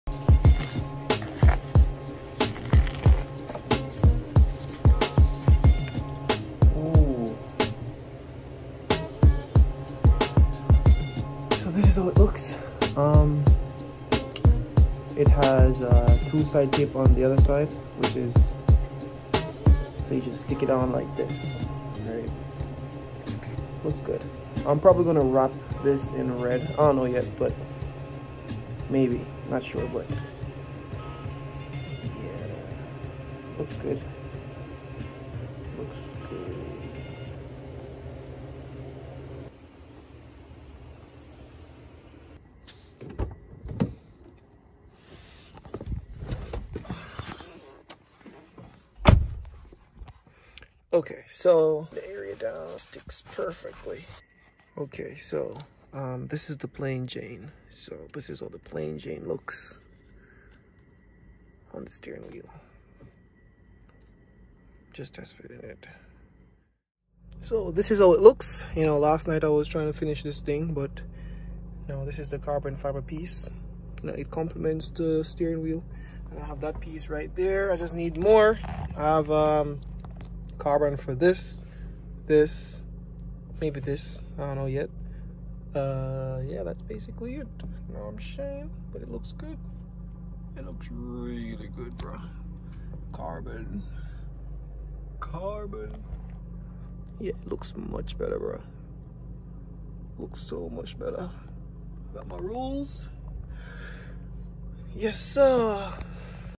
9TH GEN HONDA CIVIC SI |INSTALL *JDM* CARBON FIBRE STEERING WHEEL TRIM
INSTALLING CARBON FIBRE STEERING WHEEL TRIM | 9TH GEN HONDA CIVIC SI
 This part is from eBay , but it looks really good !!!